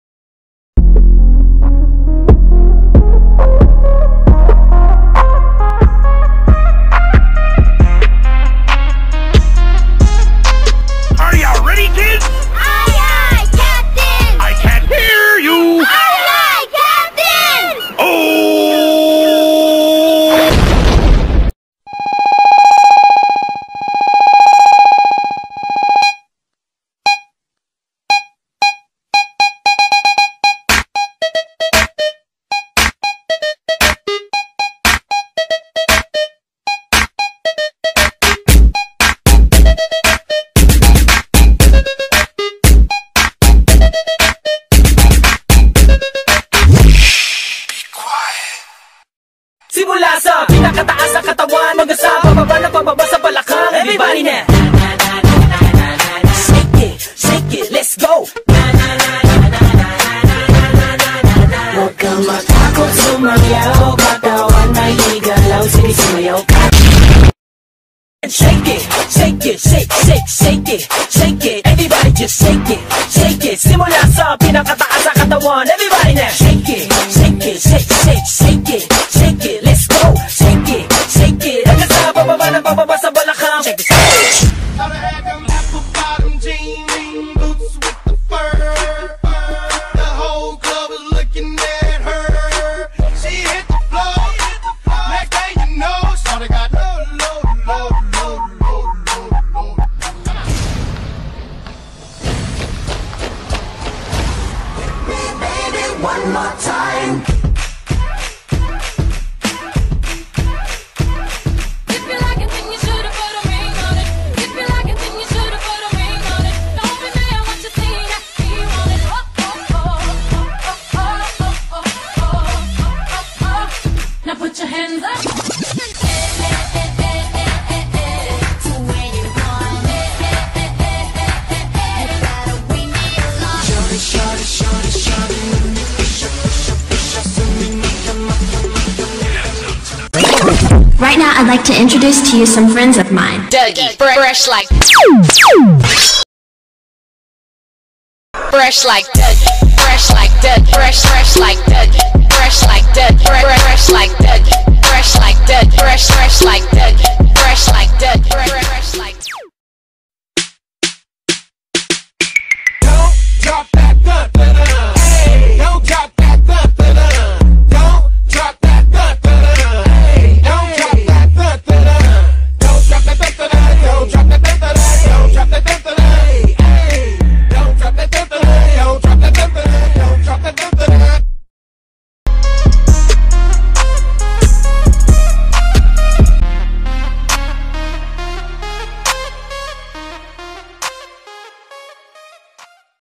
MODERN FOLK DANCE
THE ICT STUDENTS FROM PANGALANGAN NATIONAL HIGH SCHOOL S. Y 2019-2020

Choreography by:
Rolando Aquino
Jolina Rosario

Edited by:
Jolina Rosario

Videography by:
Jolina Rosario
Prencess Mae Mondares